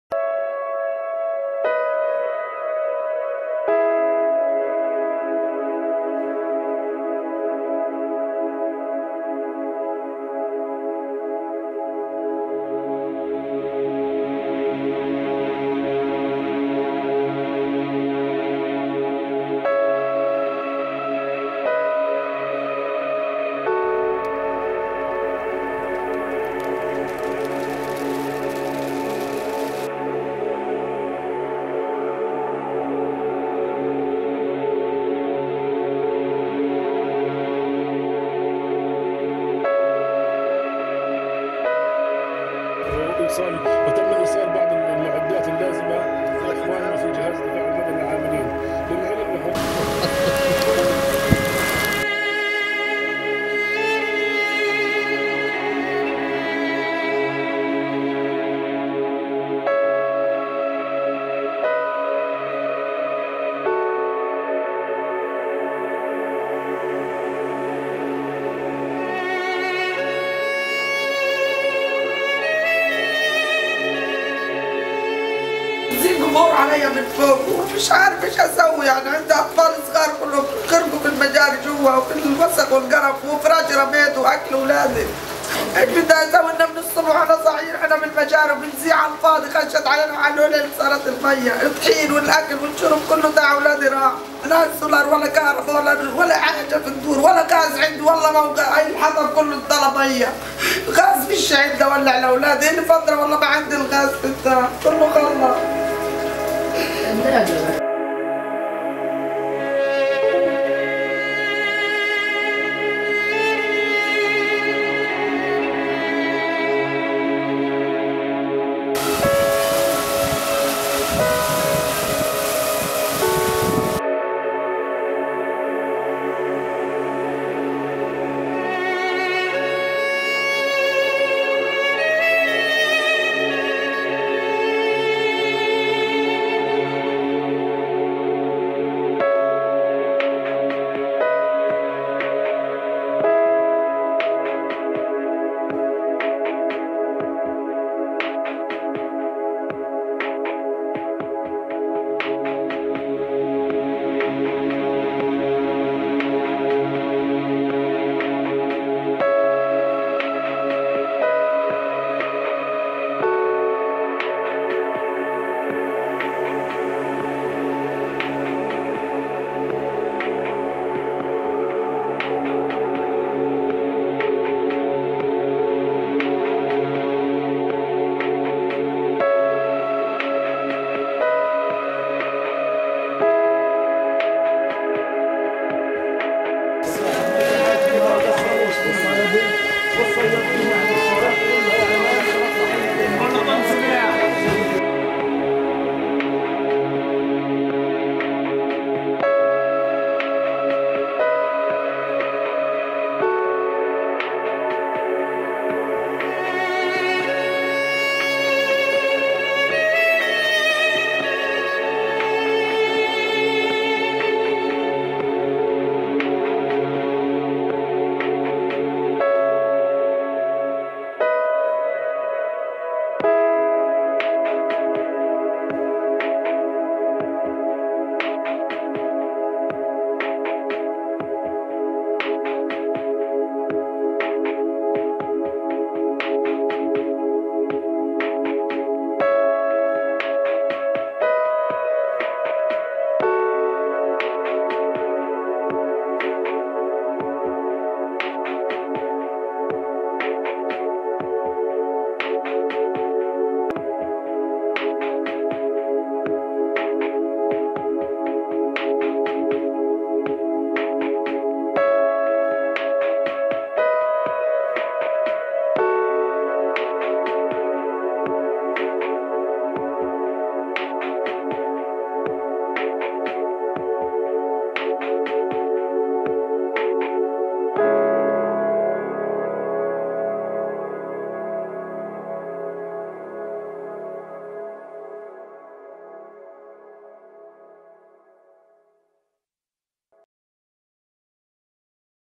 Gaza Floods
Floods and heavy rains damaged several houses and injured tens of people in the Gaza Strip during the region-wide storm that hit the unprepared coastal territory.

Islamic Relief is on the ground helping and supporting the affected people. Please help Gazans.